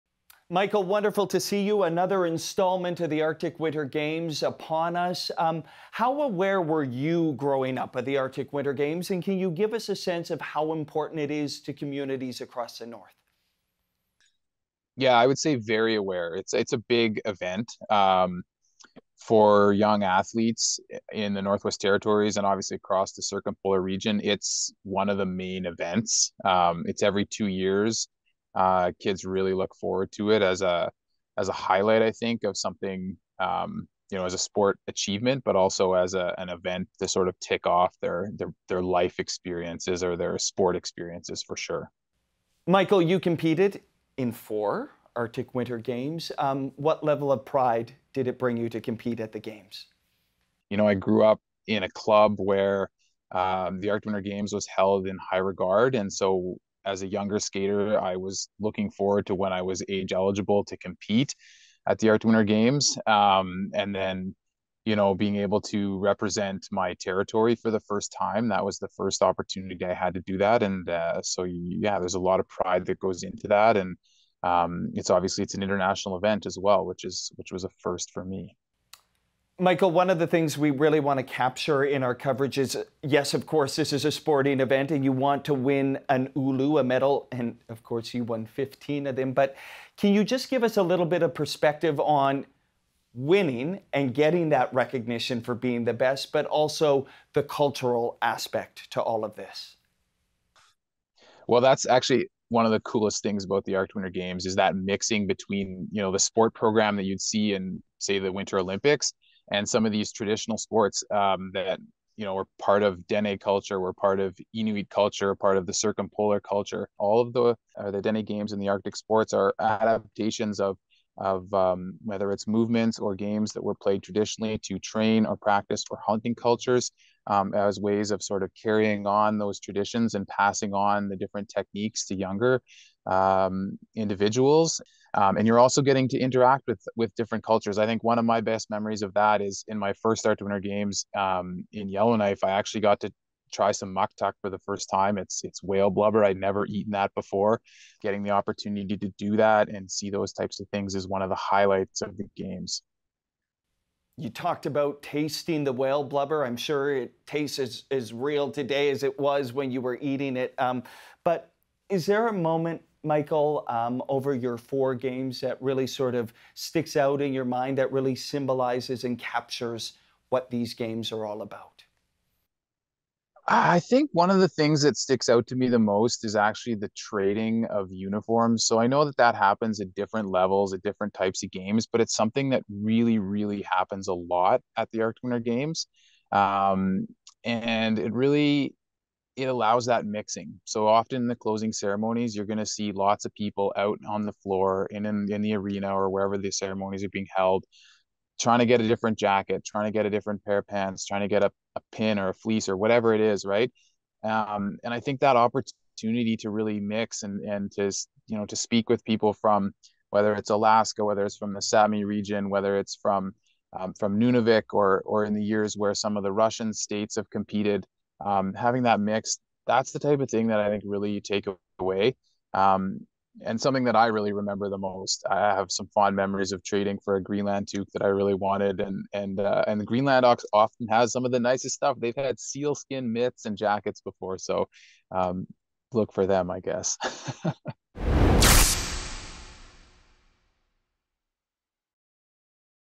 Olympian Michael Gilday reflects on competing at the Arctic Winter Games | CBC Sports
CBC Sports' Devin Heroux sits down with Canadian Olympic short track speed skater Michael Gilday to discuss the significance of the Arctic Winter Games for athletes from the circumpolar region.